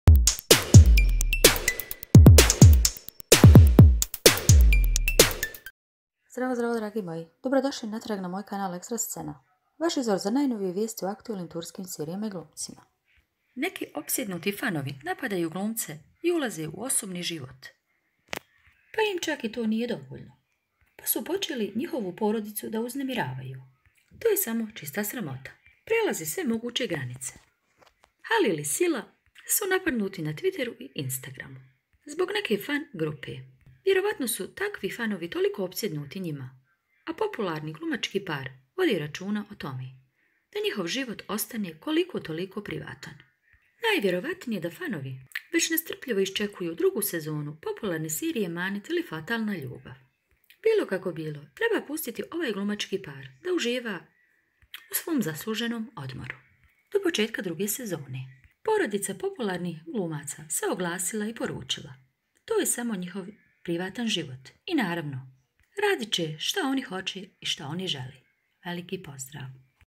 Mnogi opsjednuti fanovi prelaze sve moguce granice - Emanet 'Fatalna Ljubav'
Halil Ibrahim Ceyhan i  Sila Turkoglu nemaju mira na drustvenim mrezama zbog oosjednutih fanova.